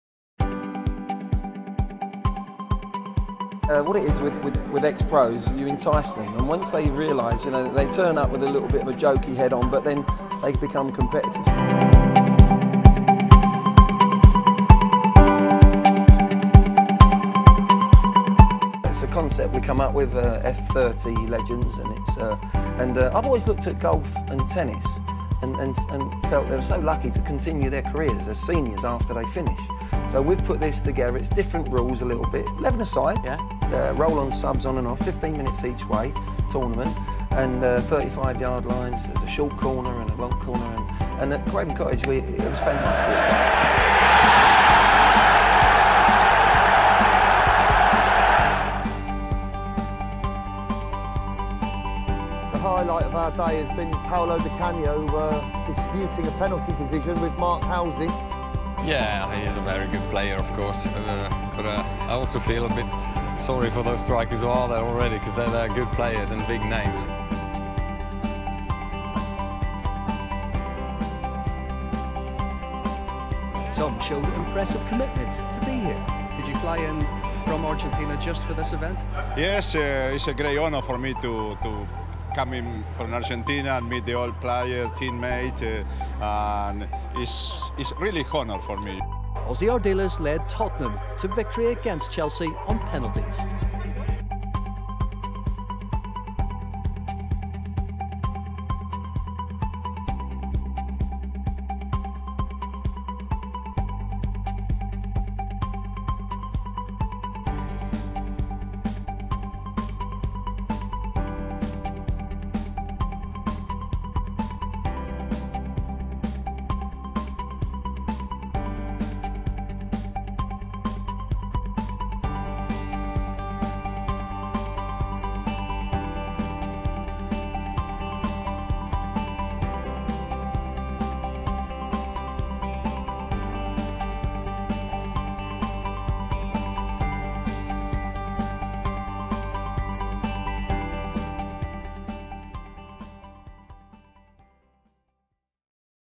Glenn Hoddle's Legends Showreel
This short video gives the background to the F30 Legends football tournament from Glenn Hoddle - Technical Director